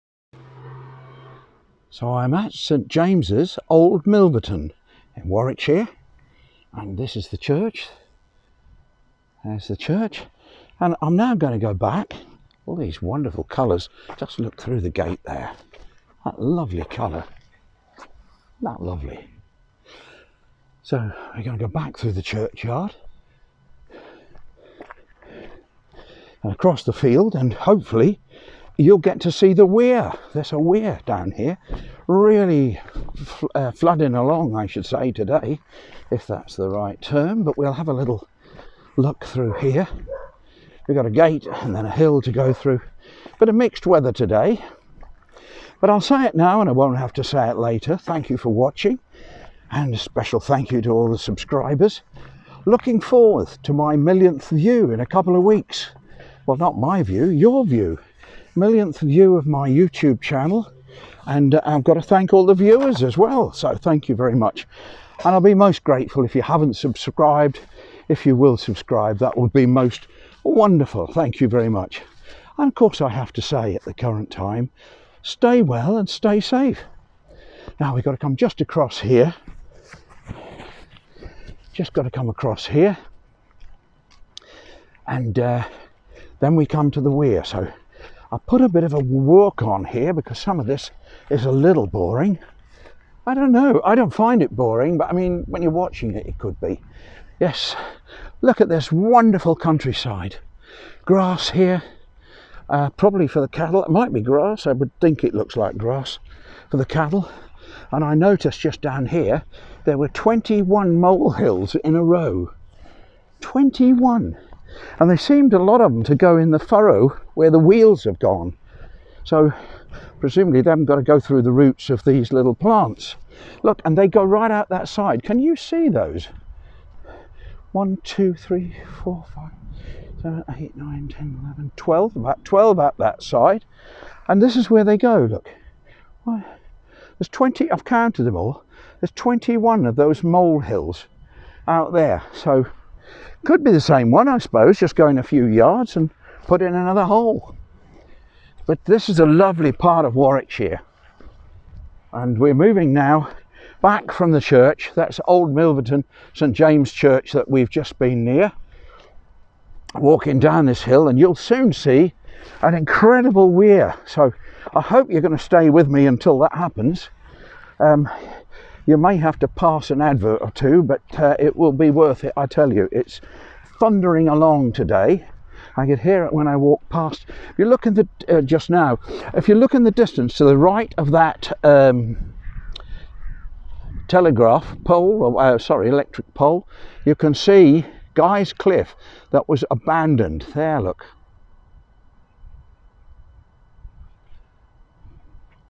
2020.11.09 Ramble 6.9km by St James, Old Milverton, in Warwick, Warwickshire. UK
Dave Hurr enjoys a 6.9km ramble by St James, Old Milverton in Warwick, Warwickshire. UK